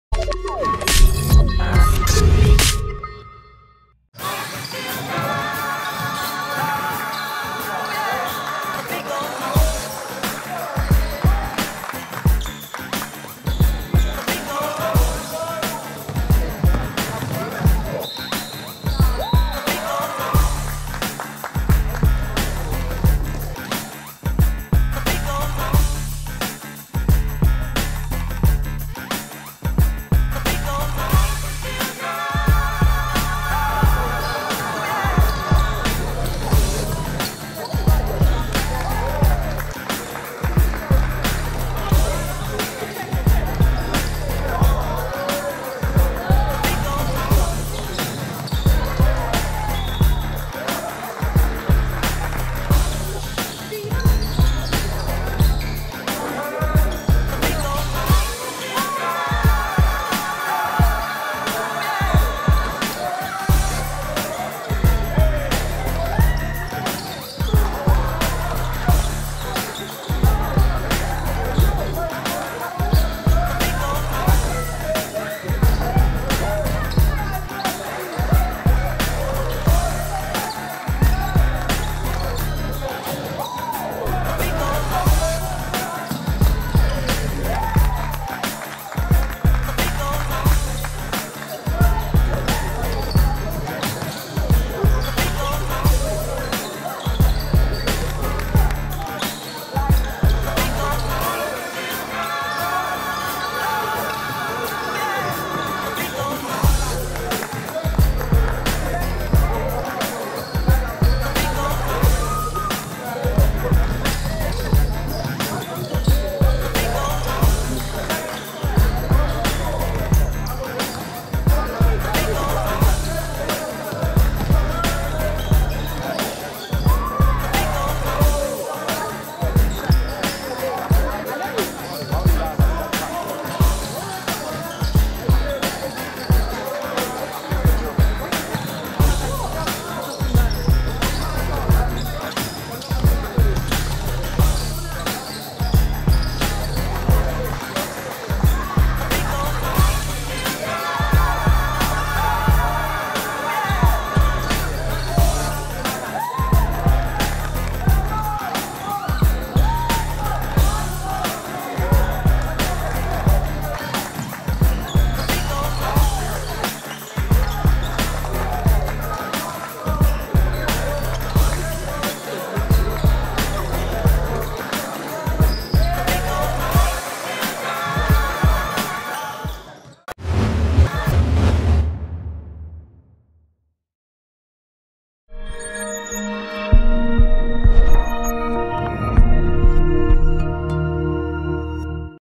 Barking Abbey vs Copleston Sixth Form - Week 4 - Highlights - 2017/18
Barking Abbey remain the only undefeated team in the EABL East Conference at 3-0 after a hard fought 75-62 win over Copleston on Wednesday at BA.

Andre Lavinier led BA with 17, as Ade Adebayo and Spyros Kapsalos added 12 each.

BA overcame a 28 point outing from Ben Winter, who caused problems throughout, and 20 points from big man Jonny Hall, who continued his impressive start to the season.

“We struggled to get going at times today,” BA Head Coach James Vear said.

“Credit to Nick and his team, they played us really well, especially Ben Winter who has improved a lot this summer. Defensively we need to improve a lot if we are to compete for the EABL title this season.”

The home side started well going up 5-0, with Daniel Delgado and Andre Lavinier setting the scoring tone
early. Copleston’s Ben Winter hit two spot up three pointers and got to the line to have eight points in the early stages, but BA led 15-13 after one.

BA stepped up defensively in the second quarter, although Jonny hall started to be a factor, drawing fouls and getting to the line. Offensively BA found some rhythm, as they scored 22 points in the period to hold a 37-28 lead at halftime.

In the second half, BA kept the margin to 10 for the majority of the contest despite Winter and Hall’s best efforts, as they reduced the lead to six with five minutes remaining before Delgado and Lavinier scored the last 10 points to secure the victory for BA.

BA return to action on November 8 at home when they face John Madejski Academy, as they will look to continue their strong start to the season, whilst Copleston return home to face BHASVIC on the same date.